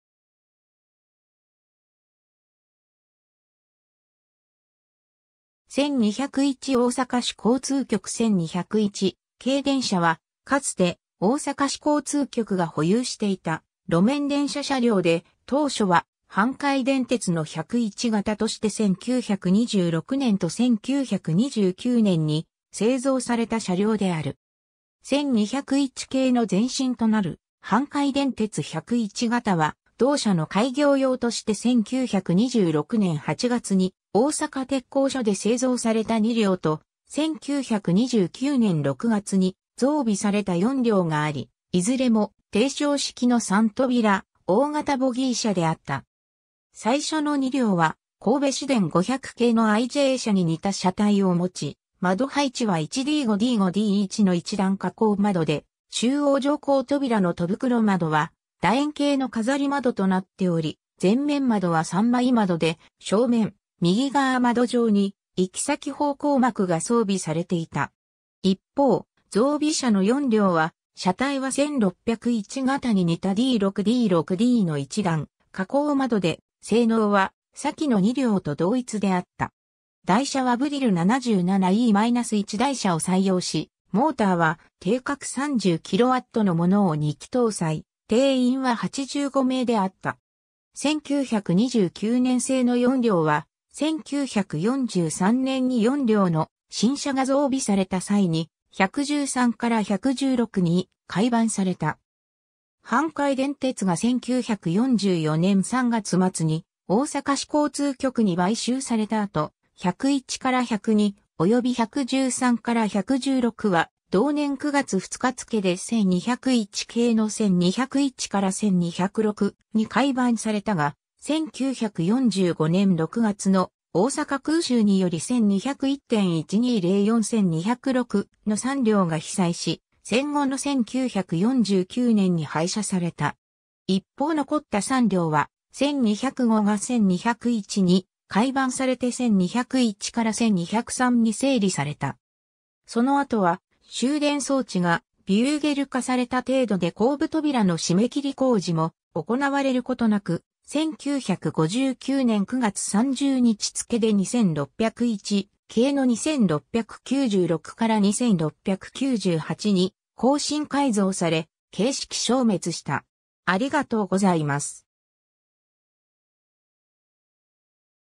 大阪市交通局1201形電車
1201 大阪市交通局1201形電車（おおさかしこうつうきょく1201がたでんしゃ）は、かつて大阪市交通局が保有していた路面電車車両で、当初は阪堺電鉄の101形として1926年と1929年に製造された車両である。
 1201形の前身となる阪堺電鉄101形は、同社の開業用として1926年8月に大阪鉄工所で製造された2両と、1929年6月に増備された4両があり、いずれも低床式の3扉大型ボギー車であった。
 最初の2両は、神戸市電500形のI・J車に似た車体を持ち、窓配置は1D5D5D1の一段下降窓で、中央乗降扉の戸袋窓は楕円形の飾り窓となっており、前面窓は3枚窓で、正面右側窓上に行先方向幕が装備されていた。
一方、増備車の4両は、車体は1601形に似たD6D6Dの一段下降窓で、性能は先の2両と同一であった。
 台車はブリル77E-1台車を採用し、モーターは定格30kWの物を2基搭載、定員は85名であった。
 1929年製の4両は、1943年に4両の新車が増備された際に、113～116に改番された。
 阪堺電鉄が1944年3月末に大阪市交通局に買収された後、101～102及び113～116は同年9月2日付で1201形の1201～1206に改番されたが、1945年6月の大阪空襲により1201・1204・1206の3両が被災し、戦後の1949年に廃車された。
一方残った3両は、1205が1201に改番されて1201～1203に整理された。
 その後は、集電装置がビューゲル化された程度で後部扉の締切工事も行われることなく、1959年9月30日付で2601形の2696～2698に更新改造され、形式消滅した。